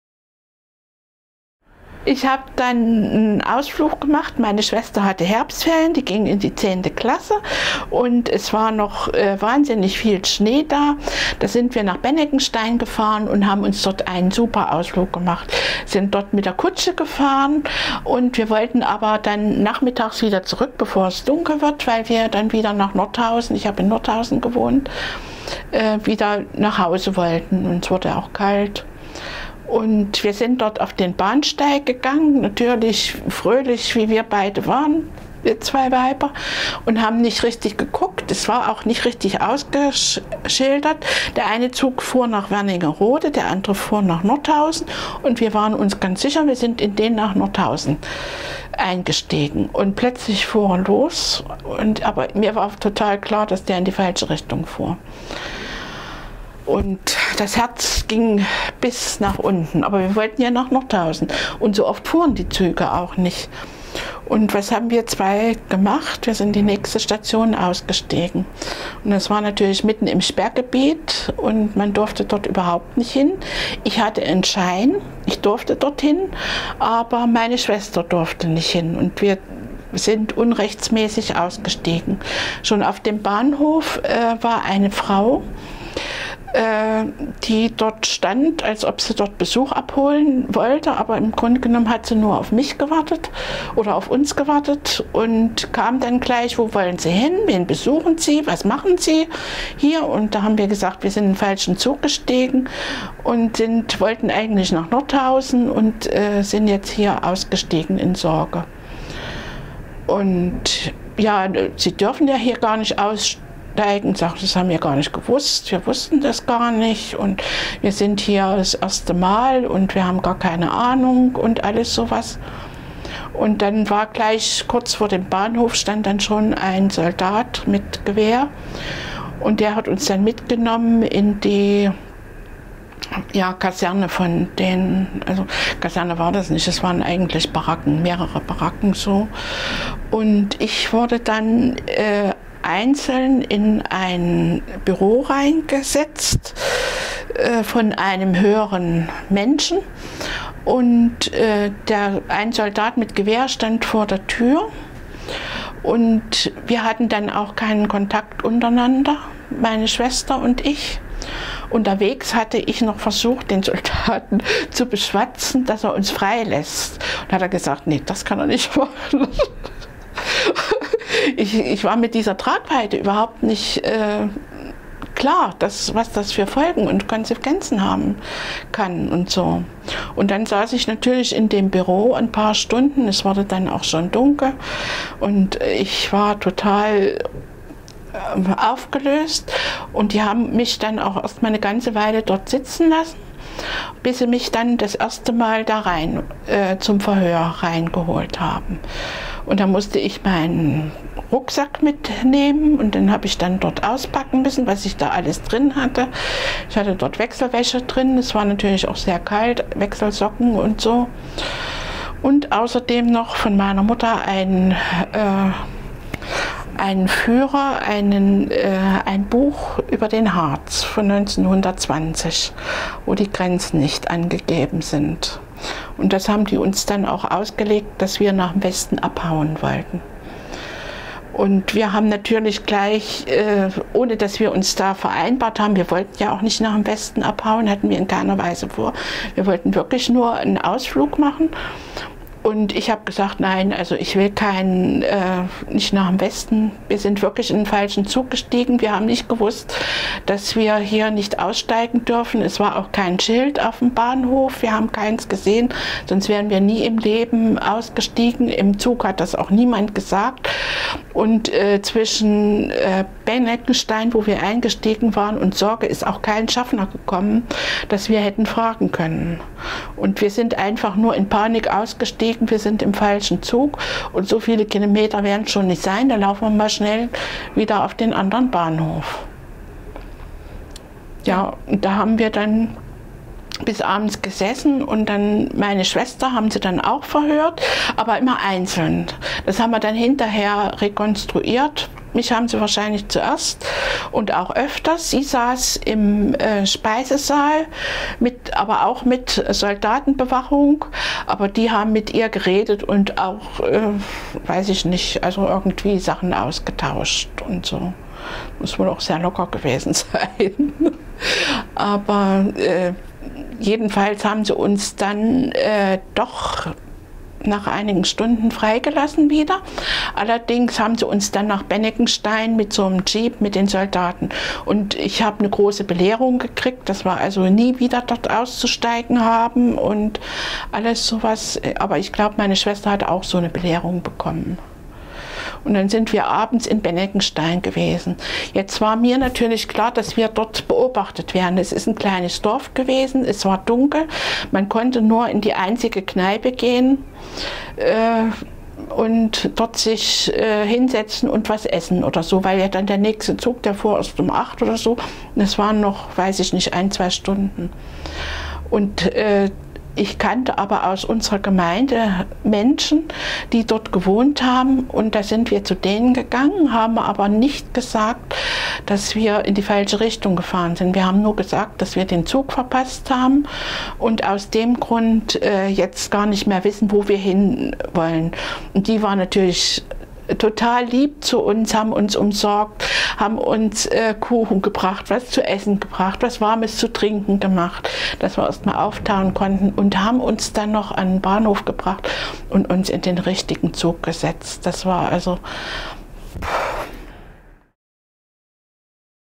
Angelika Liebrecht: Zugfahrt mit fatalen Folgen
Angelika Liebrecht arbeitete seit 1977 als Fürsorgerin im Dekanat Nordhausen. Zusammen mit ihrer Schwester machte sie einmal einen Ausflug in das nahe gelegene Benneckenstein (Harz). Auf dem Rückweg nahmen sie den erst besten Zug am Bahnhof. Zu spät bemerkten sie, dass der Zug in Richtung Bundesrepublik fuhr. Sie wurden festgenommen und der Republikflucht bezichtigt.